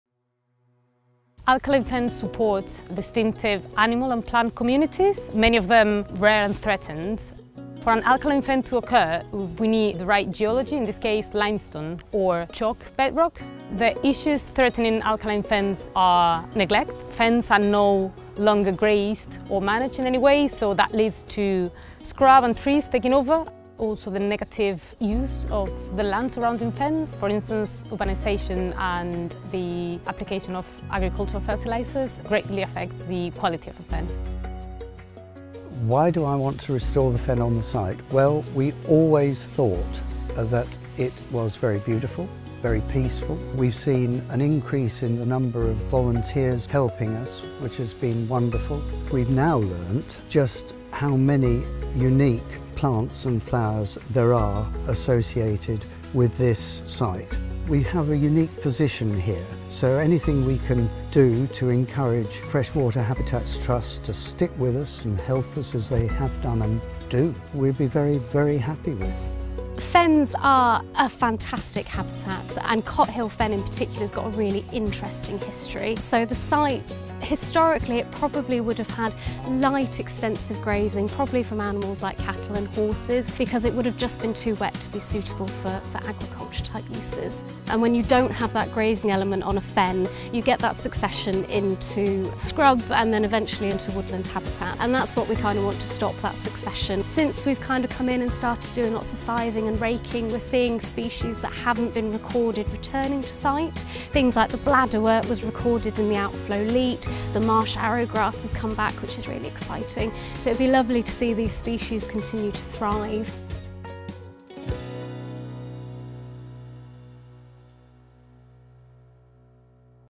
Restoring alkaline fens: the Oxfordshire-Buckinghamshire Freshwater Network
Freshwater Habitats Trust is working with partners and volunteers to restore alkaline fens in Oxfordshire.

The Oxfordshire–Buckinghamshire Freshwater Network project is part of the Nature Returns programme, led by Natural England in close partnership with the Environment Agency, Forestry Commission and RBG Kew, Wakehurst.